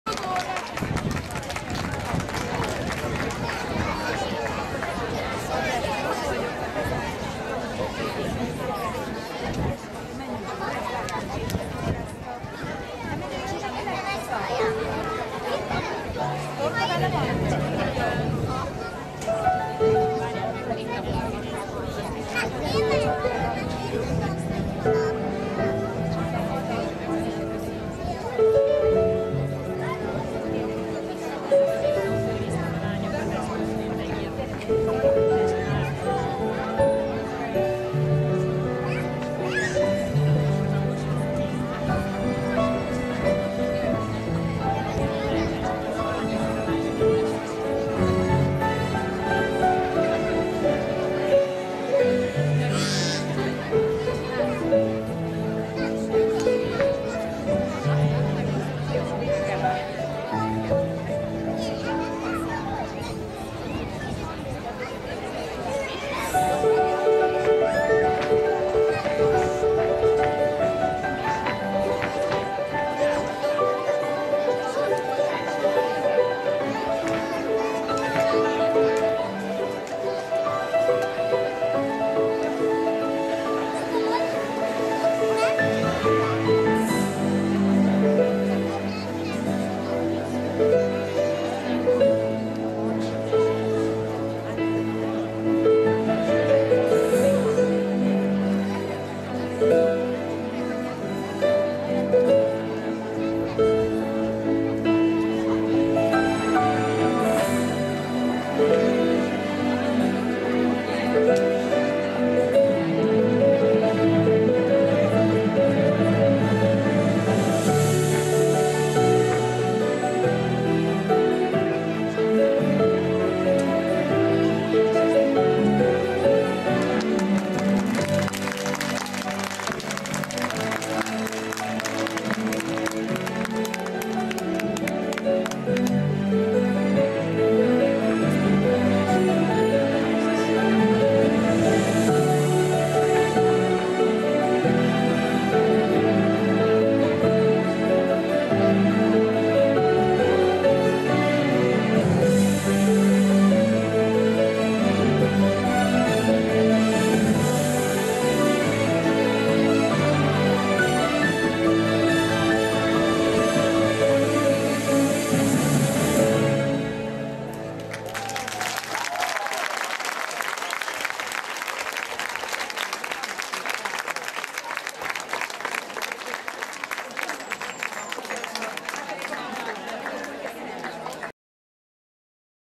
Telki Fesztivál 2011 - RG bemutató / Összhangban
A gyakorlatot bemutatják: Dalnoki Szonja, Fél Andrea, Kis Adél, Kéringer Zsófia, Kovács Ágnes, Kun Dominika, Ráduly Nóri.